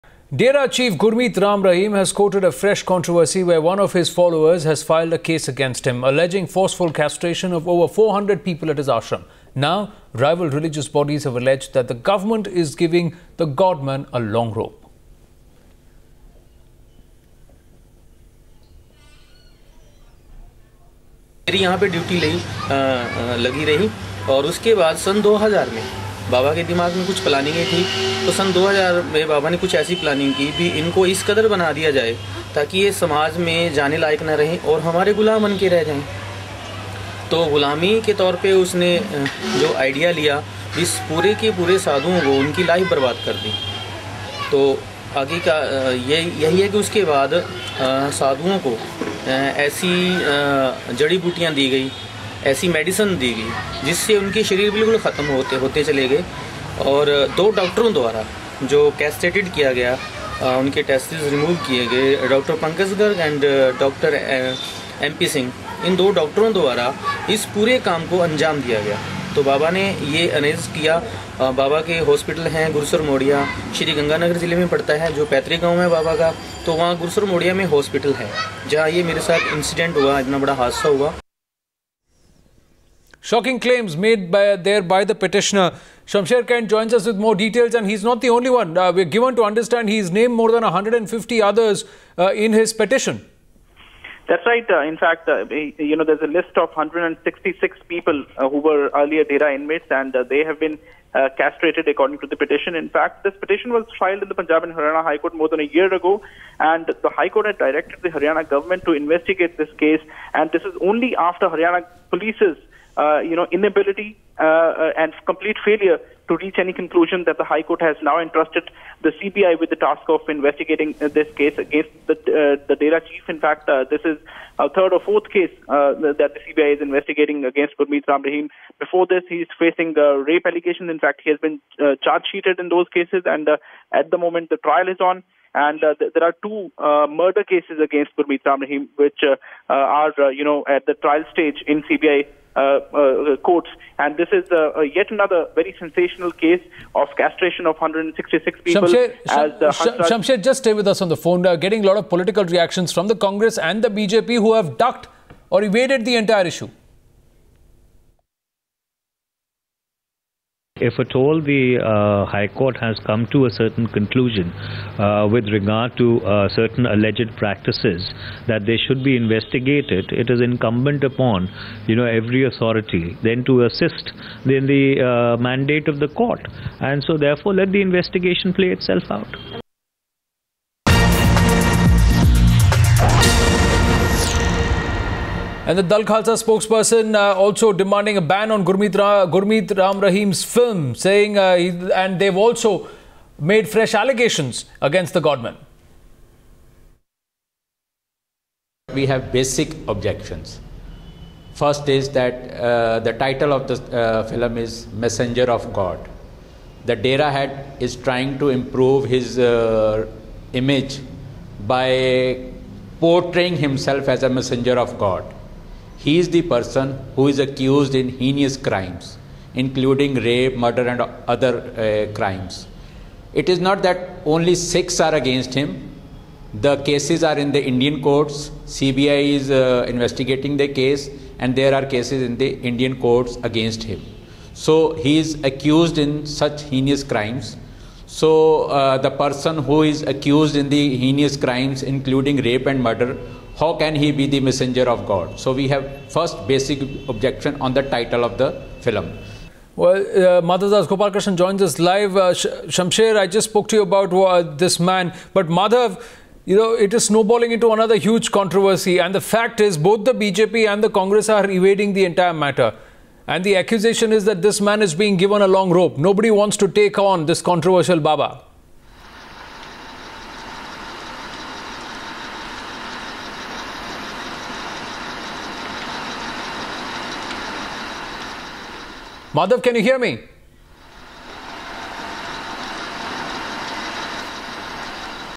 Dera Sacha Sauda Chief In Fresh Controversy
Dera Sacha Sauda chief Gurmeet Ram Rahim has courted a fresh controversy. One of his followers has filed a case against him alleging forceful castration of over 400 people at his ashram. Rival religious bodies have alleged that the government is giving the godman a long rope.